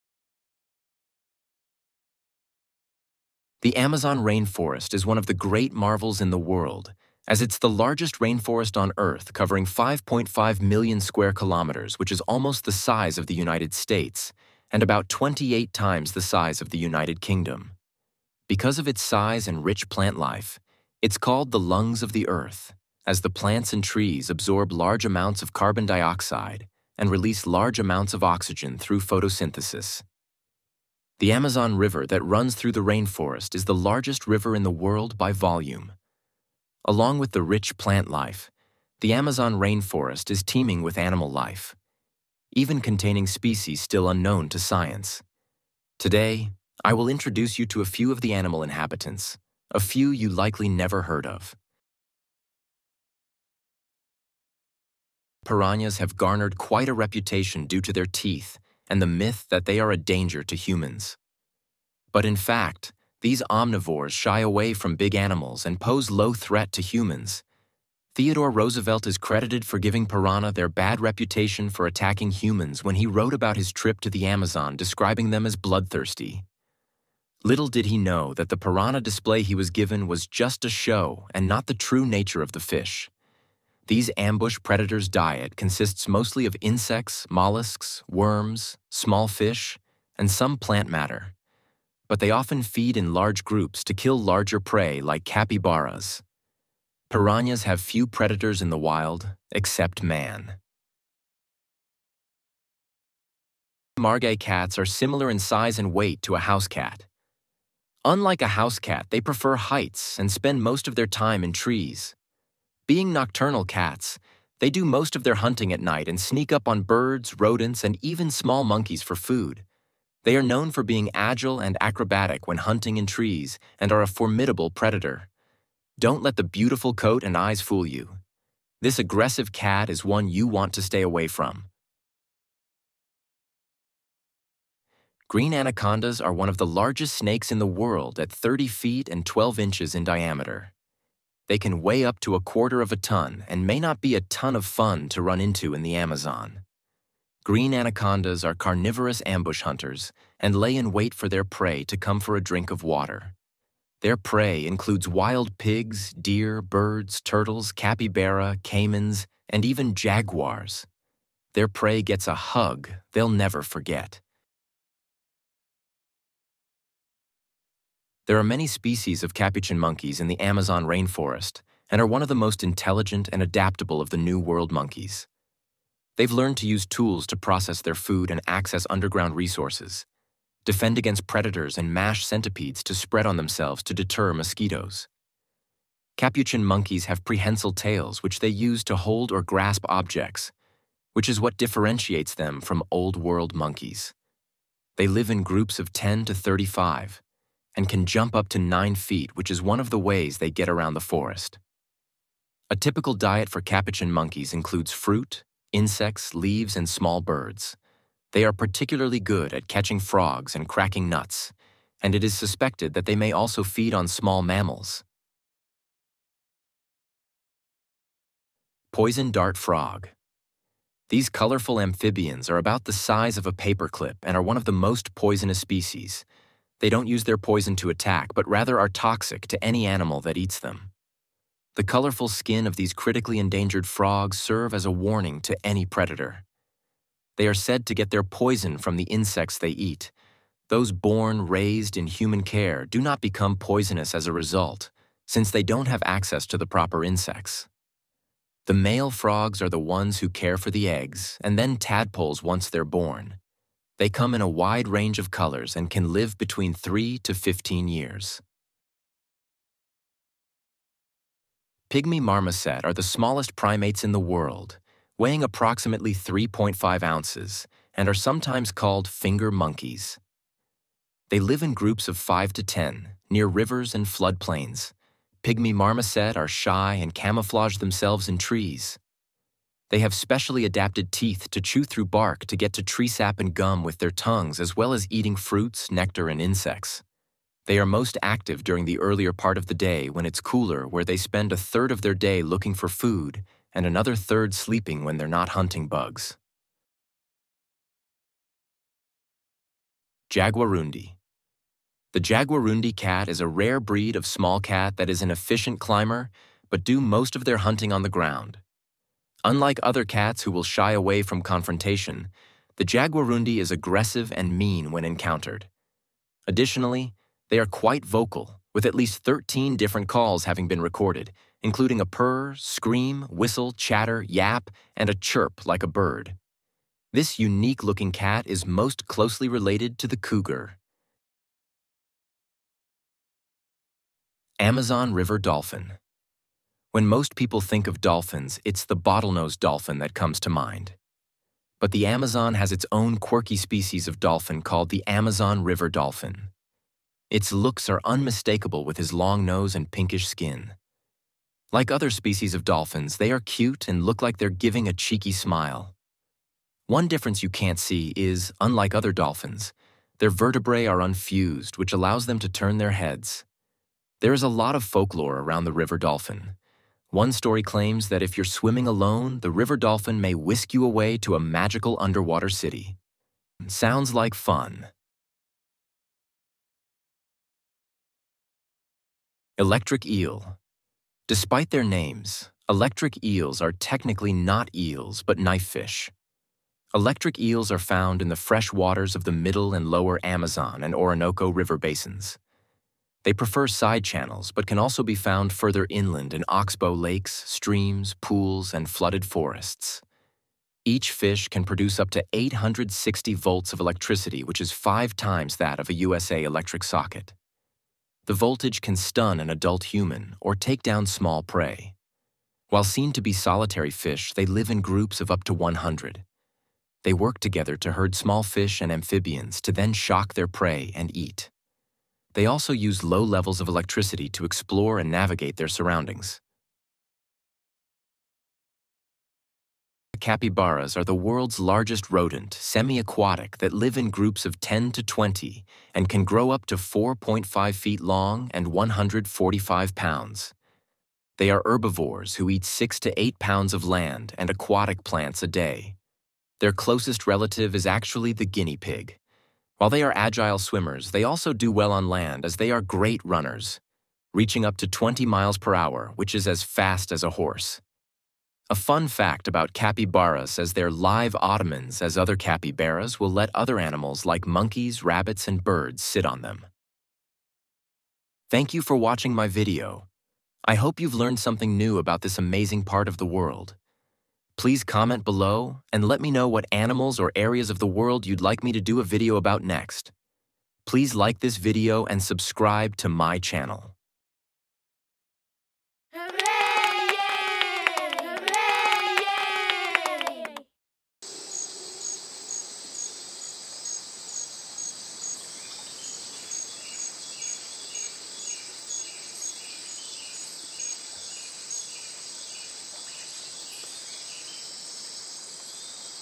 Unveiling Amazon Rainforest Creatures
Dive into the mysterious world of the Amazon Rainforest and uncover the fascinating creatures that call it home. Watch beautiful footage of these interesting animals in their natural habitat.